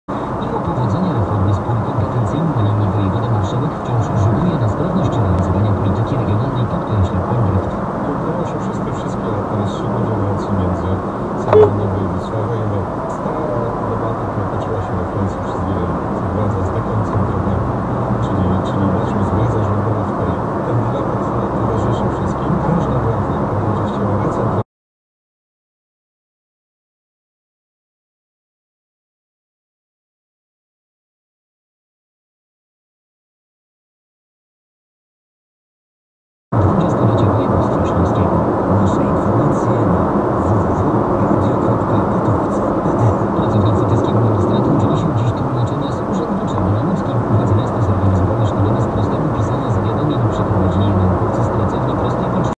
Niechlujna jazda kierowcy czerwonej mazdy
Miejsce: DK1 - Czechowice-Dziedzice
Czas: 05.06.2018, około godziny 16:00
Czasami nie wiem co jest gorsze na drodze: niechlujstwo, rozkojarzenie czy brawura.